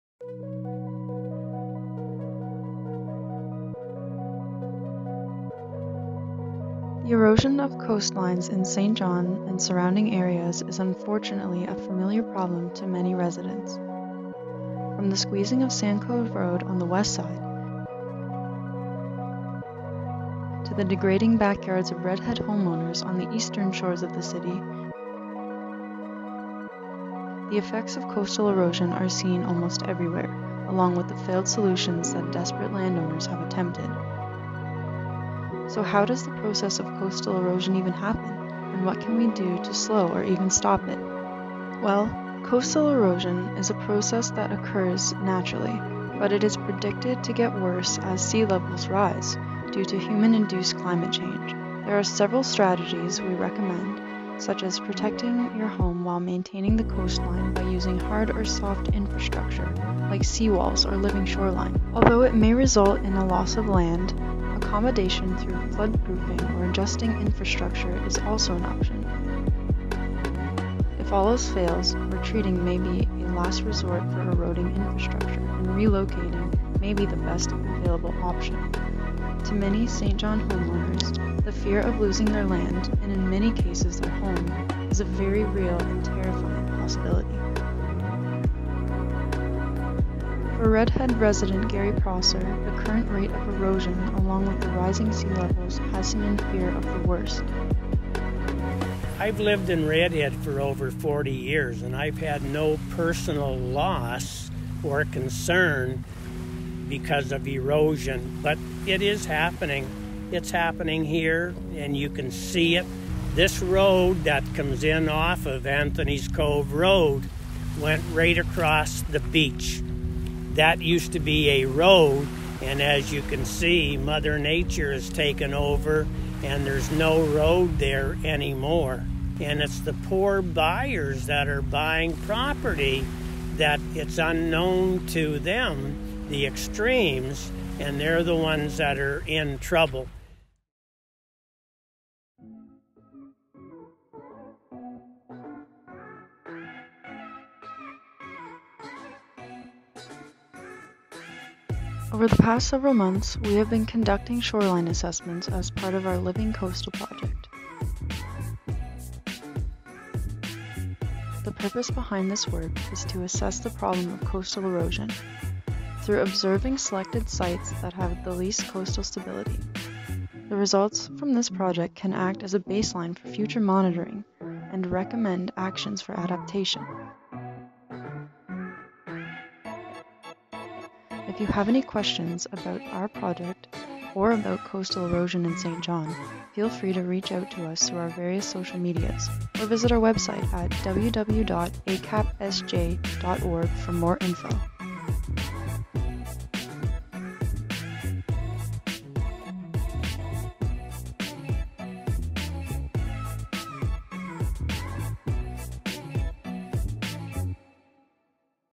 Living Coastal in Saint John, New Brunswick
This year we have been observing shoreline changes and engaging with community members who have first hand experience with coastal erosion. As this video describes, changing climate will accelerate erosion creating challenges for infrastructure, and in the worst case scenario resulting in the relocation or complete loss of structures including homes. This research intends to create a baseline of shoreline conditions for Saint John, New Brunswick that can be used to evaluate changes in stability, protection efforts, and restoration opportunities.

This project has been funded by the New Brunswick Environmental Trust Fund.

Credits:
Video creation - M. Watson
Narration - S. Braydon